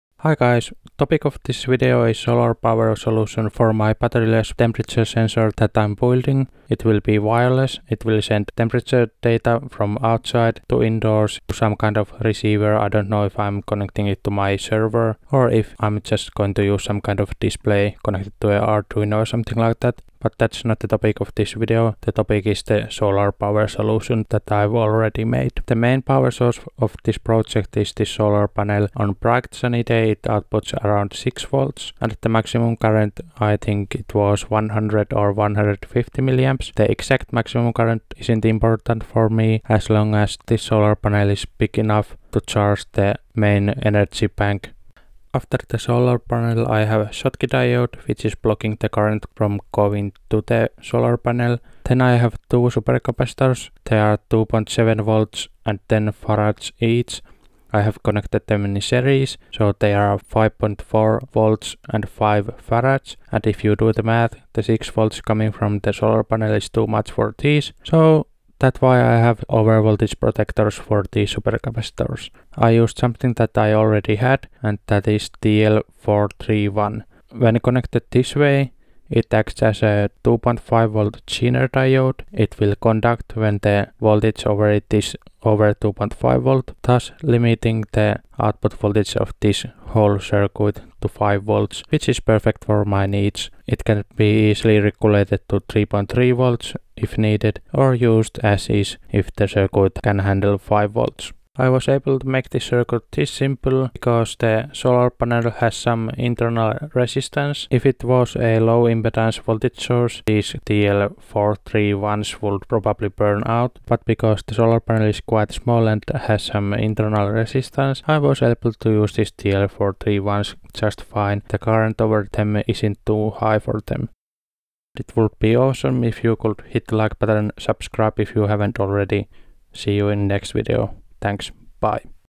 Powering (for example) Arduino with solar panel
This is quick video showing the simple solar panel based power supply I made for wireless (and batteryless!) thermometer project.
After making the first investment of buying these components, I don't need to spend money on batteries or other energy sources.

The super capacitors don't hold much energy, 5F is nothing compared to common AA(A) or 18650 batteries, but it doesn't matter. The solar panel will fully charge the capacitors during the day and stored energy will be enough to power many things over night. In my test I was able to power STM32F103, nRF24L01 and Si7051 temperature sensor from this circuit. The STM32 and nRF24L01 spent 10 minutes in power down mode, woke up, measured and sent the temperature. Avarage current consumption was in range of high tens of microamps, or maybe hundred microamps. YMMV, output of solar panels will depend highly on weather... Depending on your location, this may not be as effective way to power your projects.

The TL431 used as a over voltage protector works in my case, but with different solar panel it may not be suitable. Also I have only used this solar panel system for a week or so, long term durability isn't guaranteed.

Solar panel:
Similar super capacitors:
TL431, remember to browse and see if there are cheap listings for higher quantities: